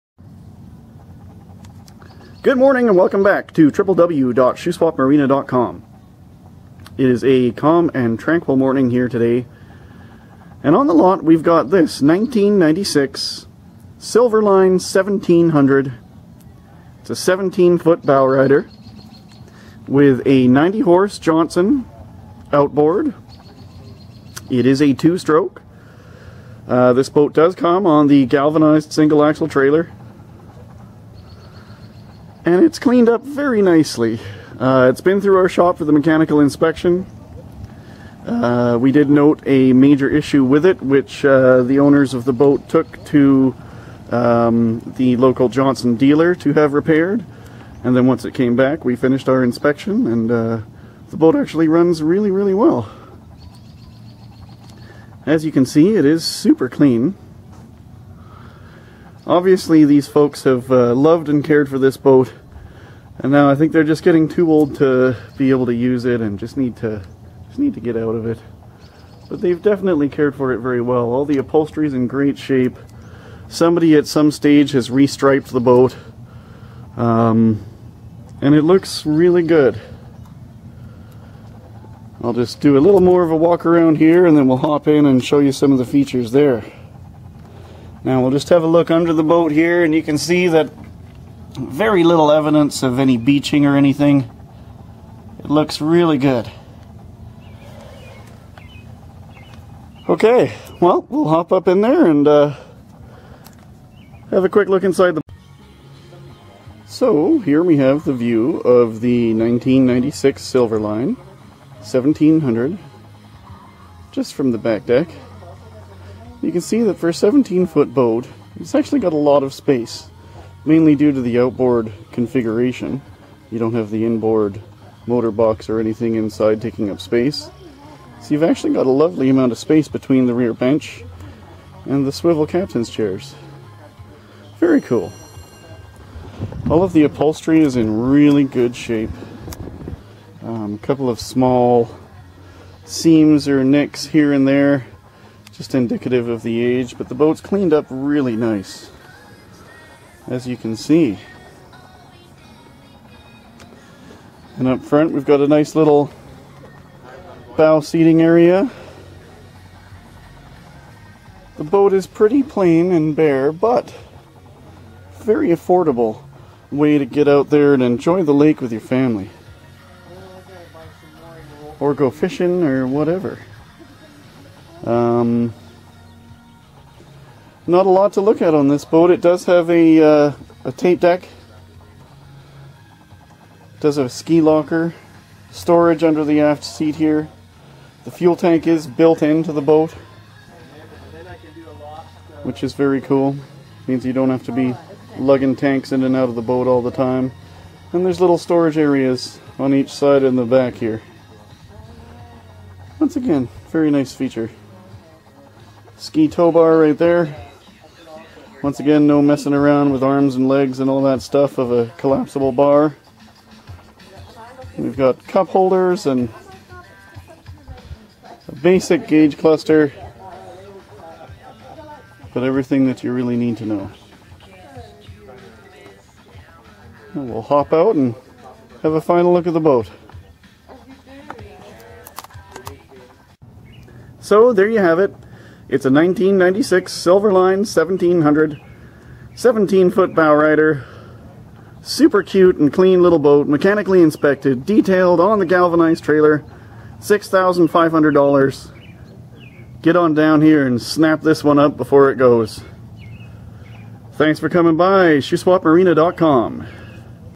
1997 Silverline 1700
This is a cute little runabout that suits an older couple or young family looking at getting into boating. The boat is in overall great shape. Mechanically inspected and detailed it has come up really clean. Appears to be a low hour boat but there is no hour meter. Runs well, and has the convertible covers you see in the pictures It has a ski tow bar and the basic safety gear inside.